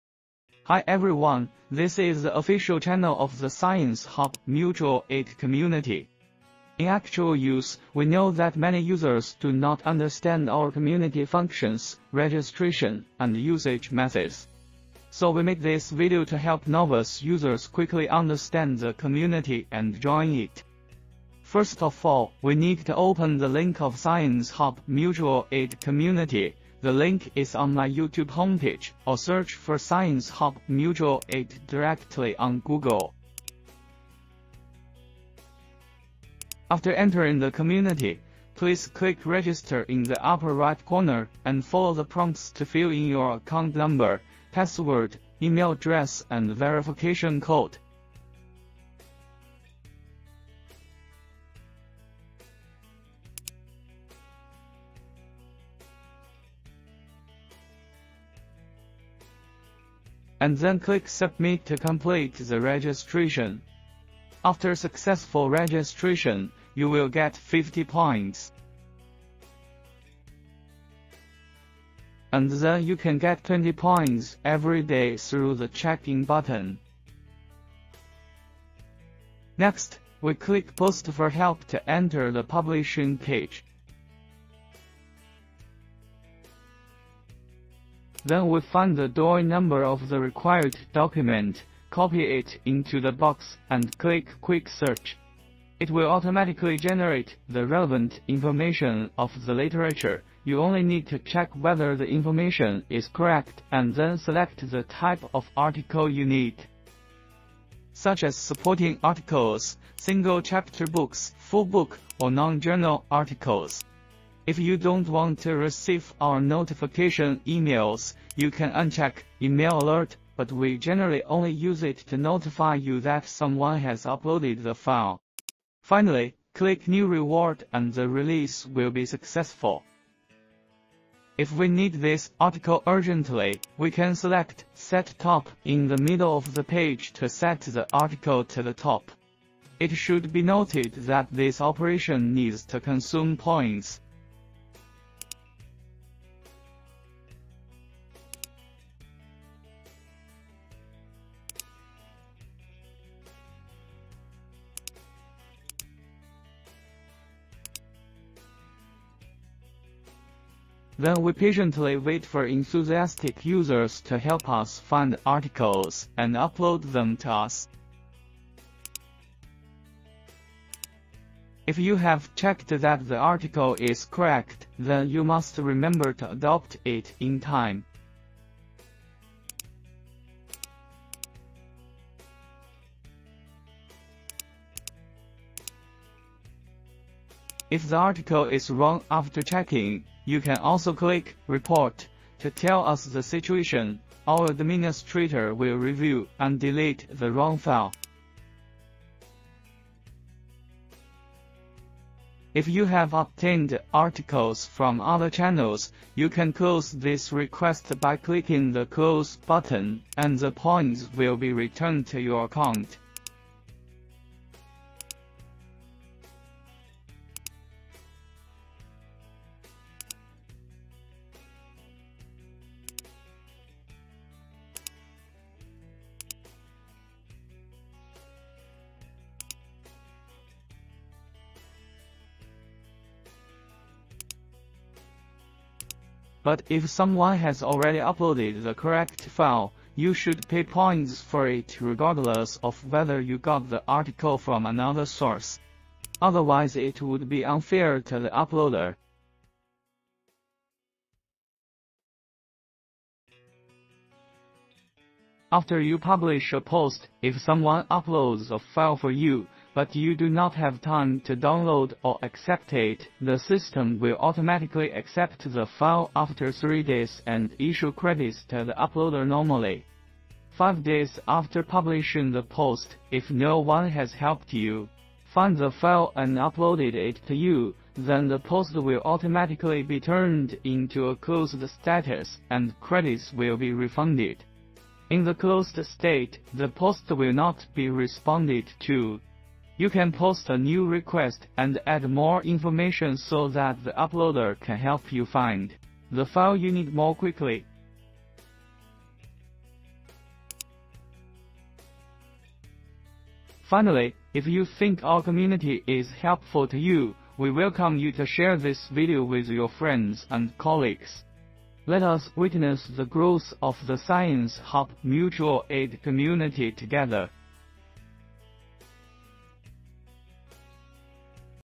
Science hub Mutual Aid community registration and usage tutorial
hi everyone, this is the official channel of the science hub mutual aid community.In actual use, we know that many users do not understand our community functions, registration, and usage methods.So we made this video to help novice users quickly understand the community and join it.

1. First of all, we need to open the link of science hub mutual aid community, the link is on my youtube homepage, or search for science hub mutual aid directly on Google.

2. After entering the community, please click Register in the upper right corner and follow the prompts to fill in your account number, password, email address and verification code, and then click Submit to complete the registration. After successful registration, you will get 50 points, and then you can get 20 points every day through the Check-in button.

3. Next, we click Post for Help to enter the publishing page, then we find the DOI number of the required document, copy it into the box, and click Quick Search. It will automatically generate the relevant information of the literature, you only need to check whether the information is correct, and then select the type of article you need, such as supporting articles, single chapter books, Full books, or non-journal articles, note: if the type is not Correct, admins have the right to close this post. If you don't want to receive our notification emails, you can uncheck "E-mail Alert", but we generally only use it to notify you that someone has uploaded the file. Finally, click New Reward and the release will be successful.

4. If we need this article urgently, we can select "set top" in the middle of the page to set the article to the top. It should be noted that this operation needs to consume points.

5. Then we patiently wait for enthusiastic users to help us find articles and upload them to us. If you have checked that the article is correct, then you must remember to adopt it in time. If the article is wrong after checking, you can also click "Report" to tell us the situation, our administrator will review and delete the wrong file.

6. If you have obtained articles from other channels, you can close this request by clicking the close button, and the points will be returned to your account. But if someone has already uploaded the correct file, you should pay points for it regardless of whether you got the article from another source. Otherwise it would be unfair to the uploader.

7. After you publish a post, if someone uploads a file for you, but you do not have time to download or accept it, the system will automatically accept the file after 3 days and issue credits to the uploader normally. 5 days after publishing the post, if no one has helped you find the file and uploaded it to you, then the post will automatically be turned into a closed status and credits will be refunded. In the closed state, the post will not be responded to. You can post a new request and add more information so that the uploader can help you find the file you need more quickly.

8. Let us witness the growth of the Science hub Mutual Aid community together!